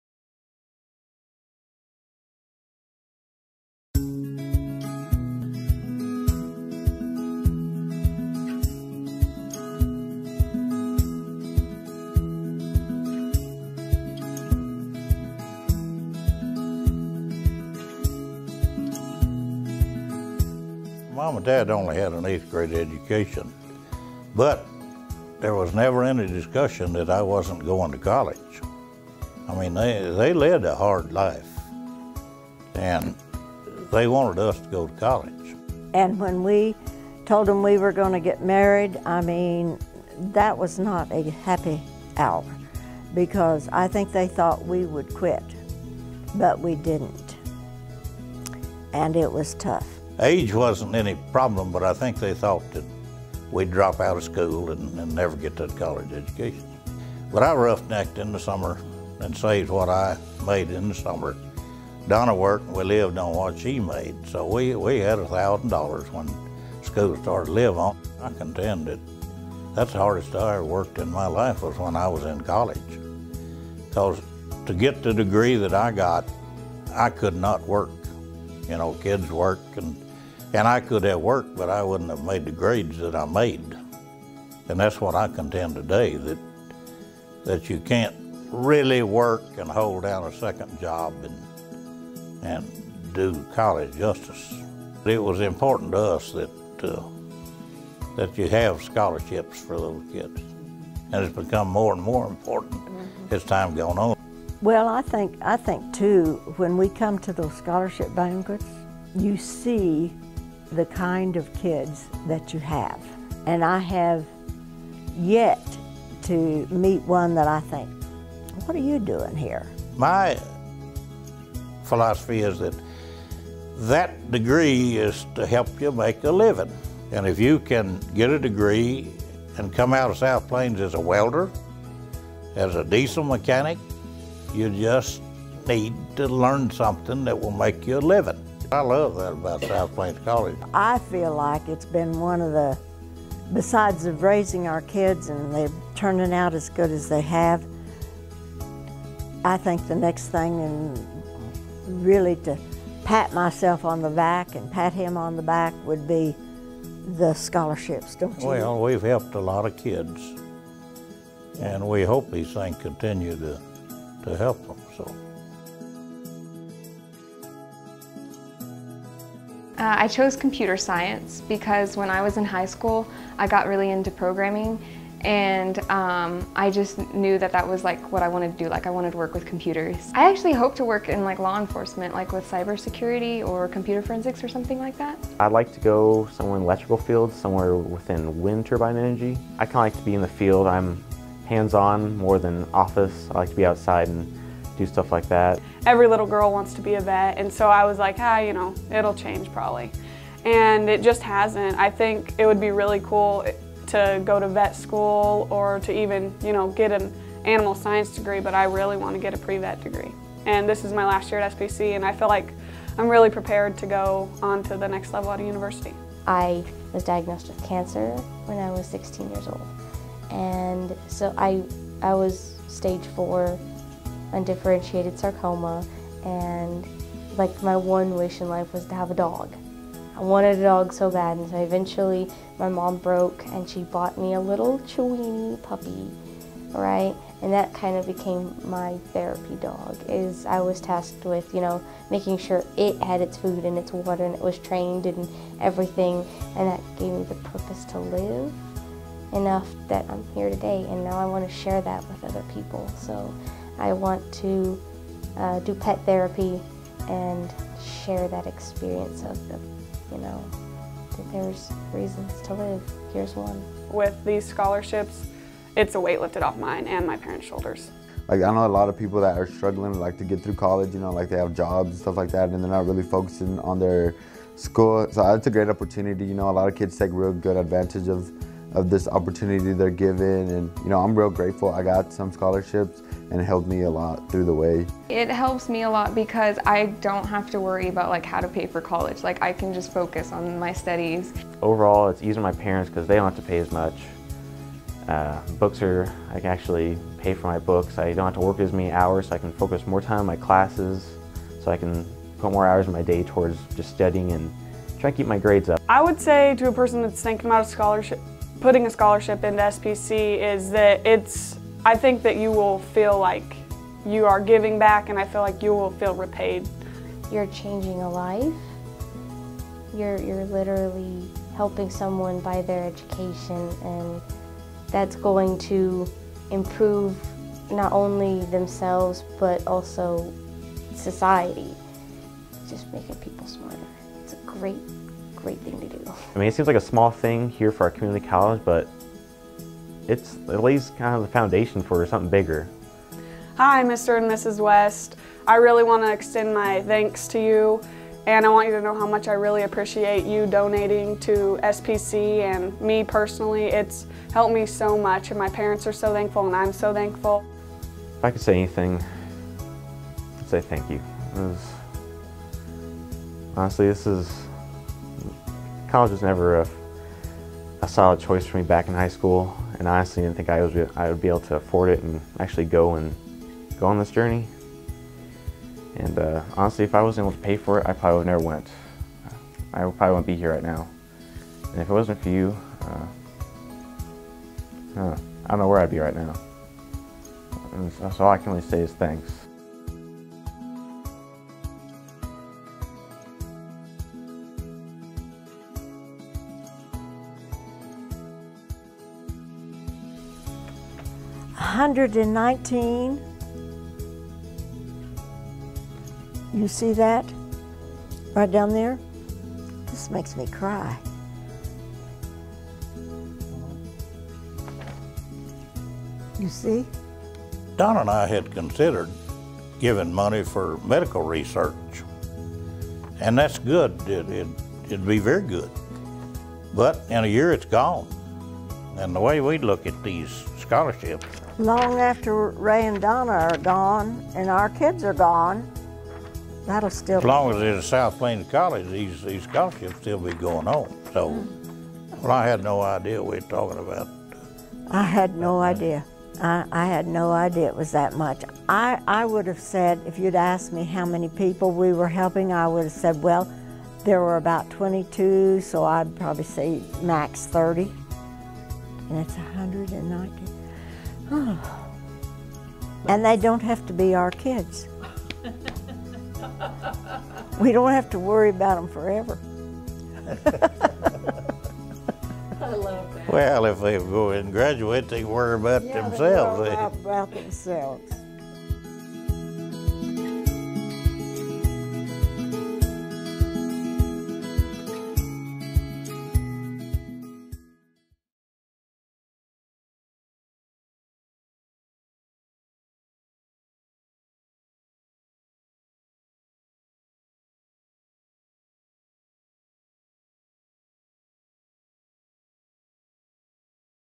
SPC 2018 Pacesetters Ray and Donna West
South Plains College honors Ray and Donna West for their philanthropy, service, and leadership.